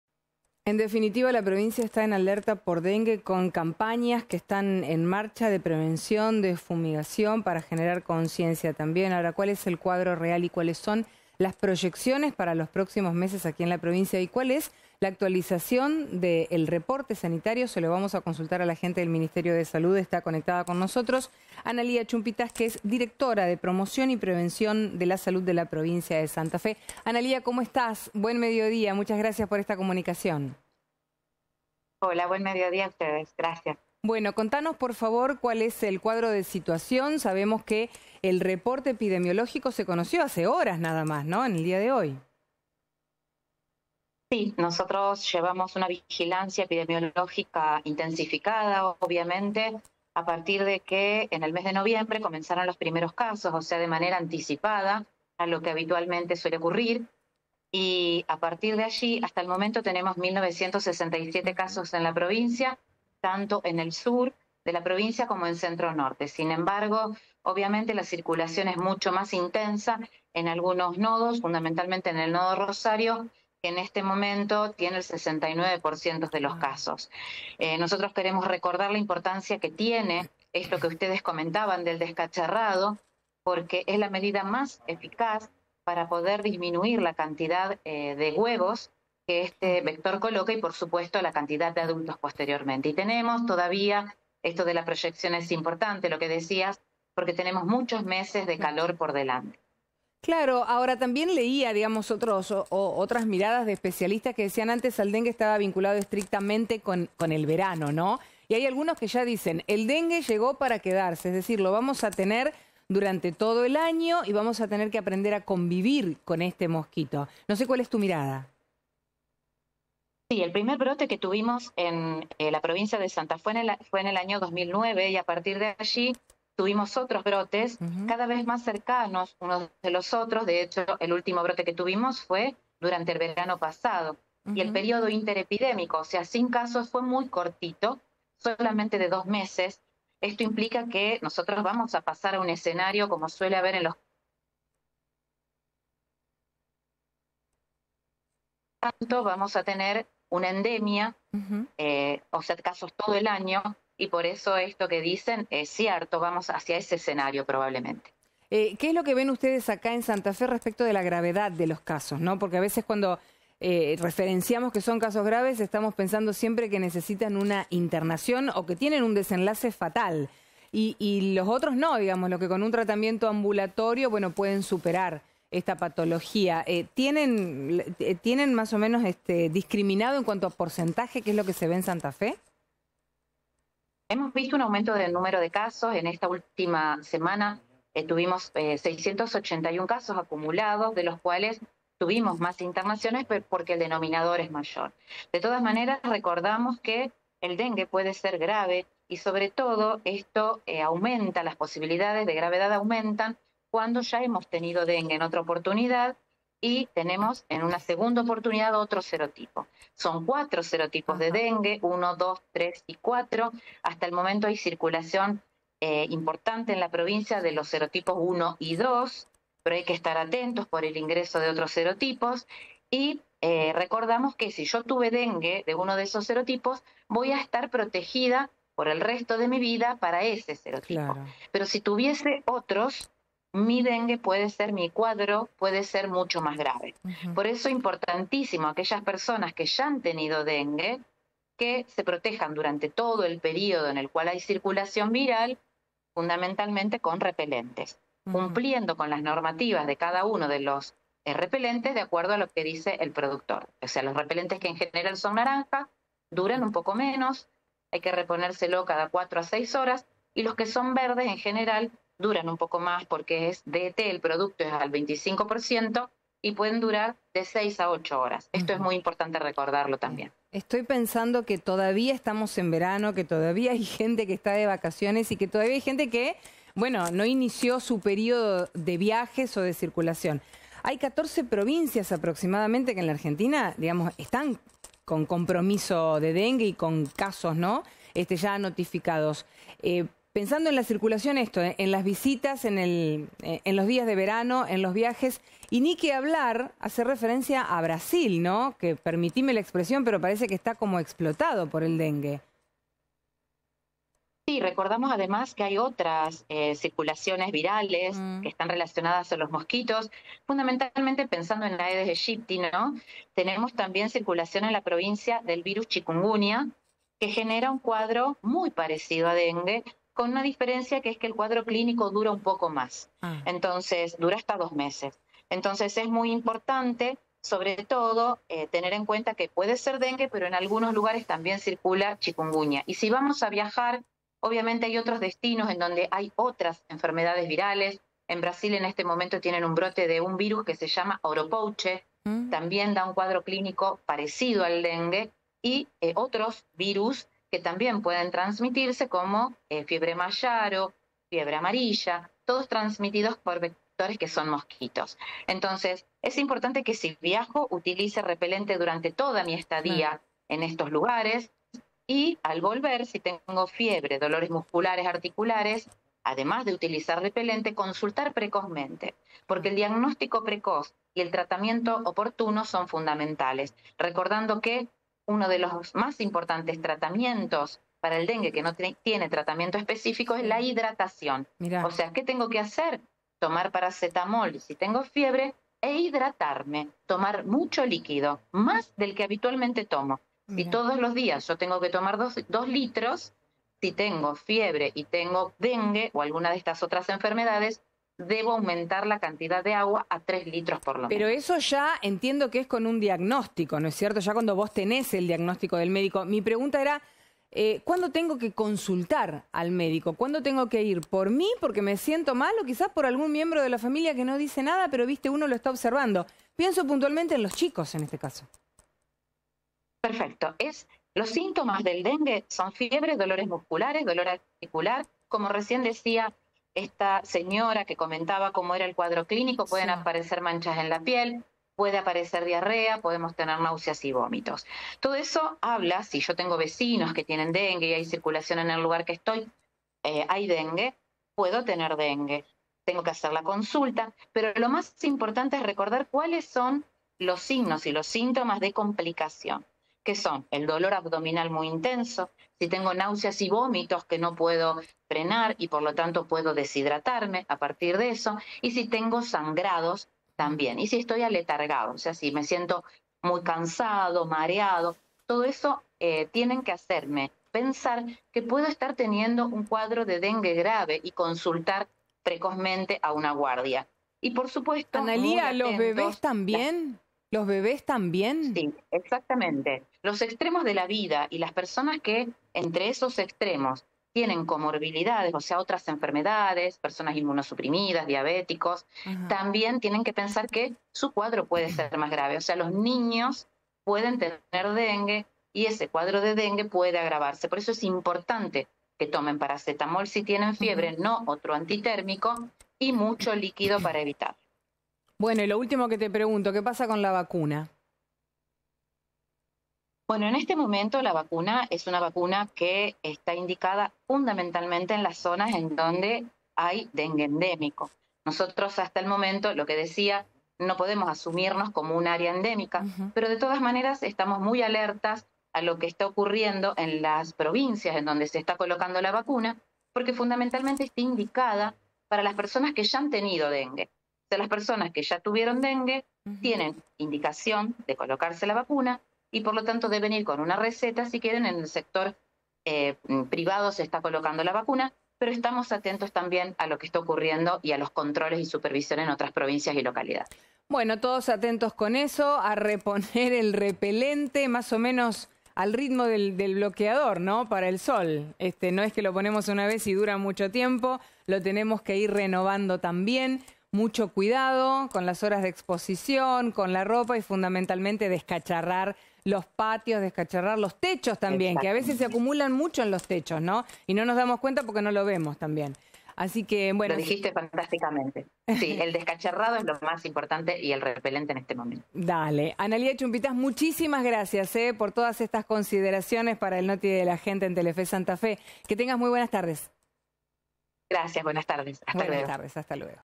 Se registraron 681 nuevos casos de dengue en la provincia
Entrevista con Analía Chumpitaz, directora de promoción y prevención de la provincia.